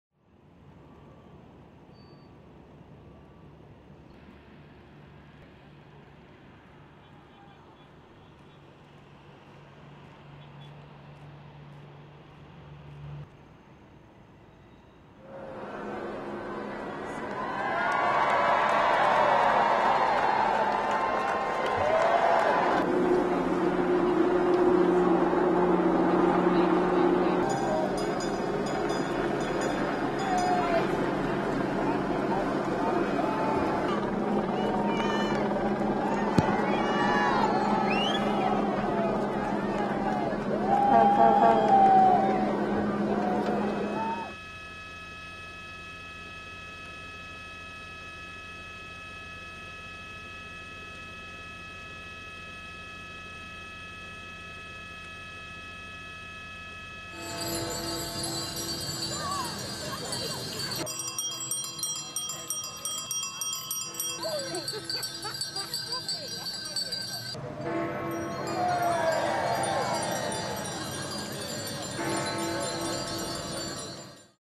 Олимпия уты Лондонда сәфәрен тәмамлый
Британия буйлар 12 мең чакрым узганнан соң Олимпия уты бүген ачылыш тантанасына берничә сәгать кала Лондонның Олимпия стадионына китерелде.